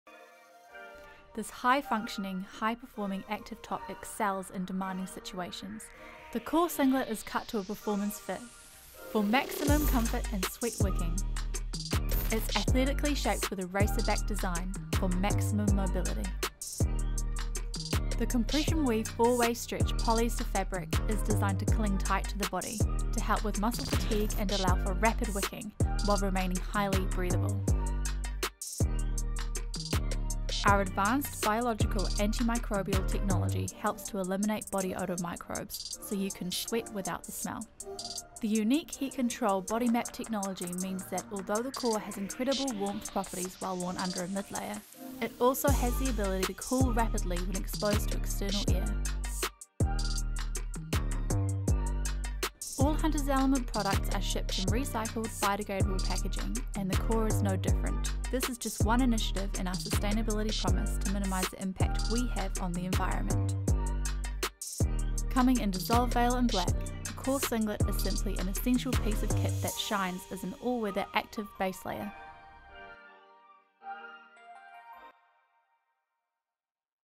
CORE Singlet Womens
A high functioning, high performing active top for demanding situations. The CORE Singlet is cut to a performance fit, for maximum comfort and maximum sweat-wicking. The compression weave is designed to cling tight to the body to help with muscle fatigue and rapid wicking. The design has been athletically shaped, so the high wicking fabric can sit tight to the body in order to absorb and disperse as much sweat as possible, all the while allowing maximum movement in the high movement areas. The unique heat control technology means that although the Core has incredible warmth properties while worn under a mid-layer, it also has the ability to cool rapidly when exposed to external air. The Core should be used in temperatures ranging from 5C to 15C for maximum performance. For colder conditions, the CORE+ range is recommended.

Flatlock seams
4-way stretch polyester, highly breathable and high wicking fabric
Racerback for maximum mobility
Muscle compression design for increased stamina
Body mapping technology for ultimate temperature and moisture regulation
All-weather base layer for temperatures 5°C to 15°C